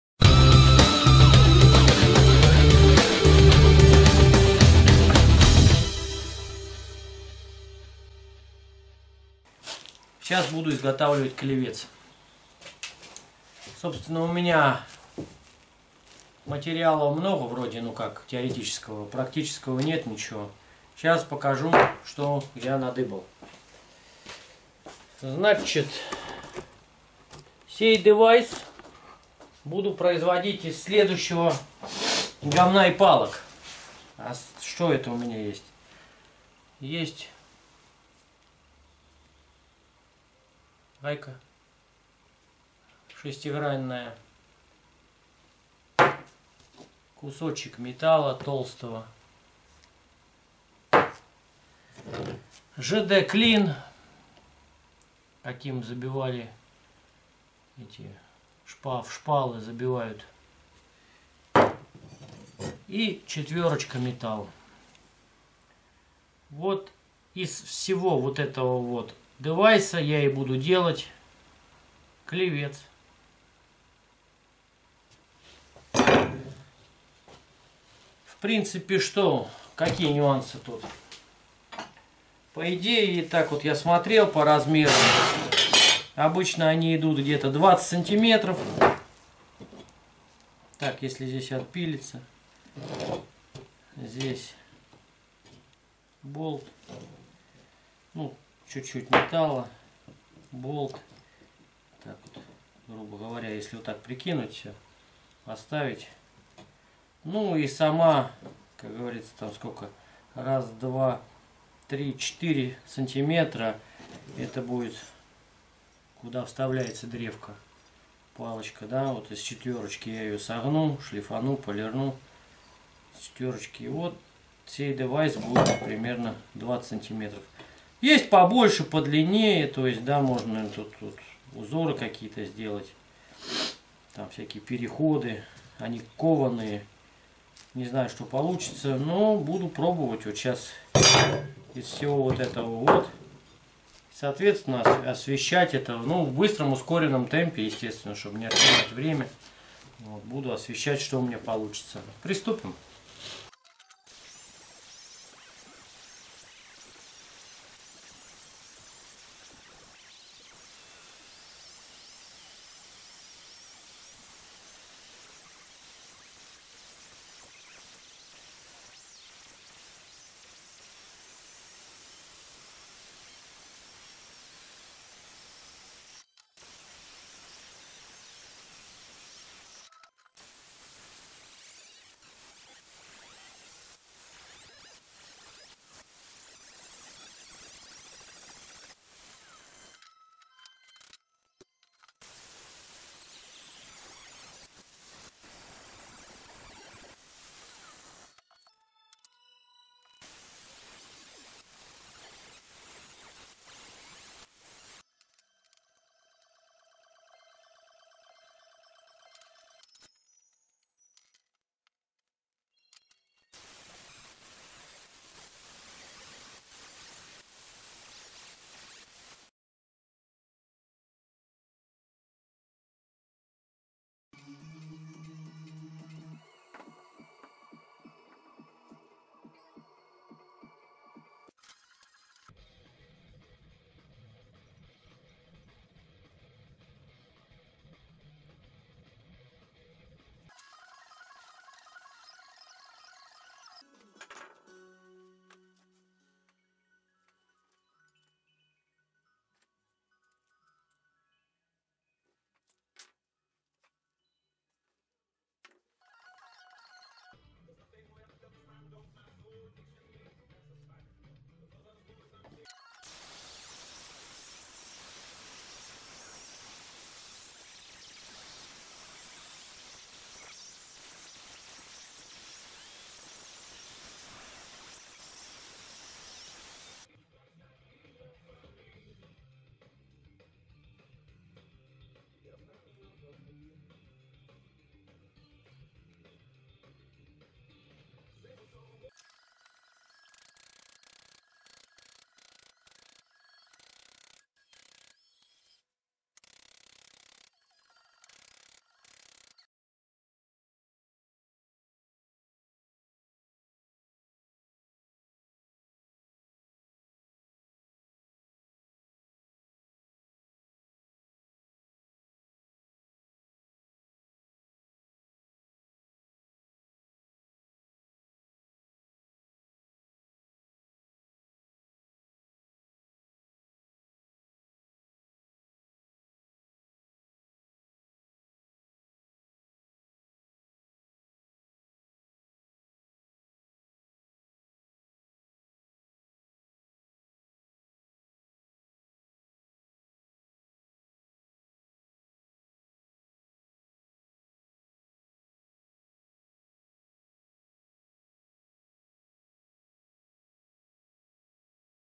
Клевец
Клевец. 4 партия закончена и сегодня я сделал клевец из того хлама который хранился у меня в мастерской.Смотрите ролик я там подробно рассказал и показал как я его изготовил. Всем удачи и творческих успехов.